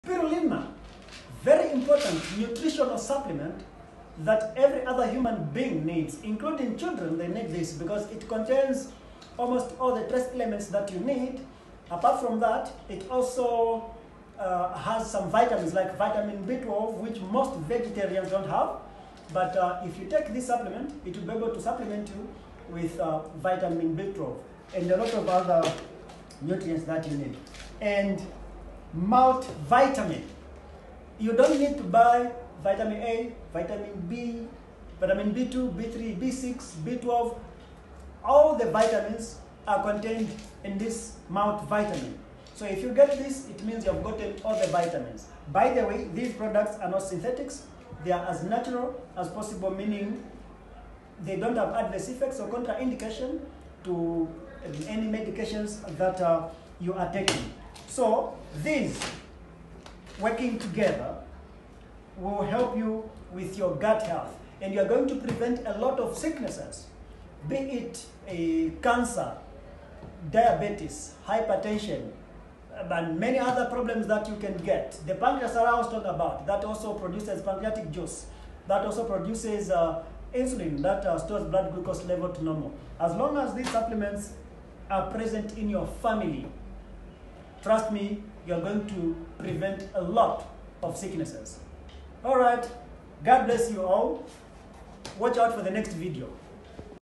Dr Lackson on gut health
Dr Lackson in Zambia training on the Imsyser deep intestinal cleanse and sundry health products